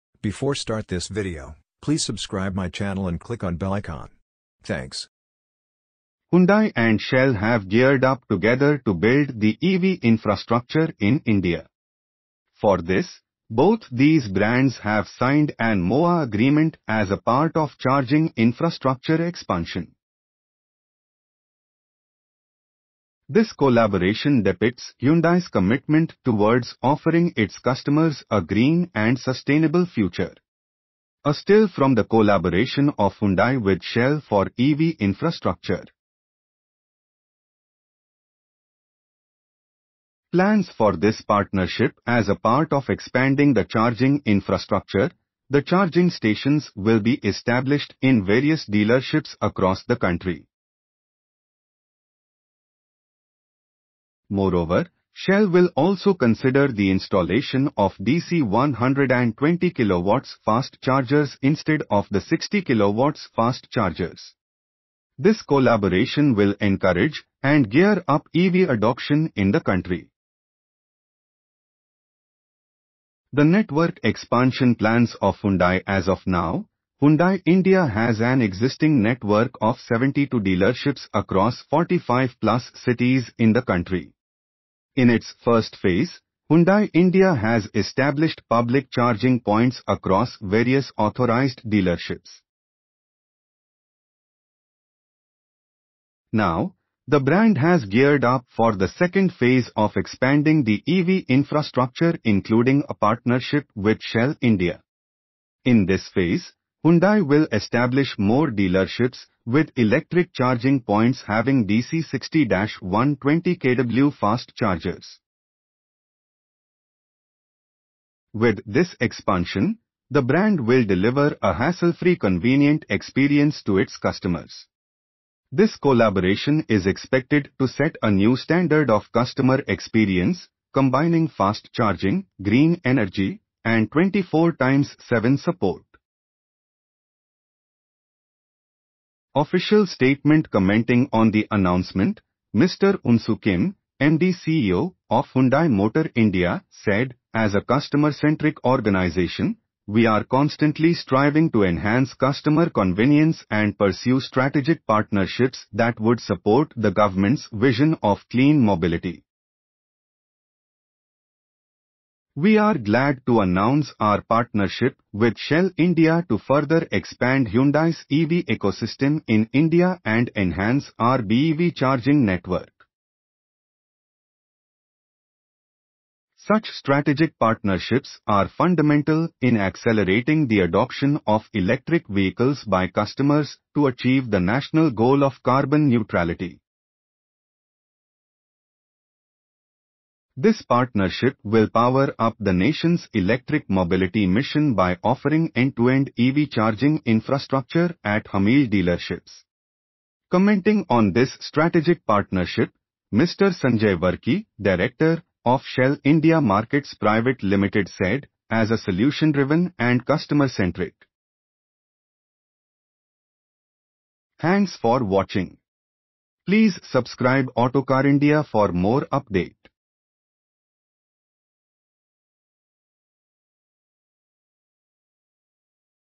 Hyundai India Ties Up with Shell for EV Infrastructure
Hyundai and Shell have geared up together to build the EV infrastructure in India. For this, both these brands have signed an MoA Agreement as a part of charging infrastructure expansion. This collaboration depicts Hyundai’s commitment towards offering its customers a green and sustainable future. A still from the collaboration of Hyundai with Shell for EV infrastructure. Plans for this partnership As a part of expanding the charging infrastructure, the charging stations will be established in various dealerships across the country. Moreover, Shell will also consider the installation of DC 120 kW Fast chargers instead of the 60 kW Fast...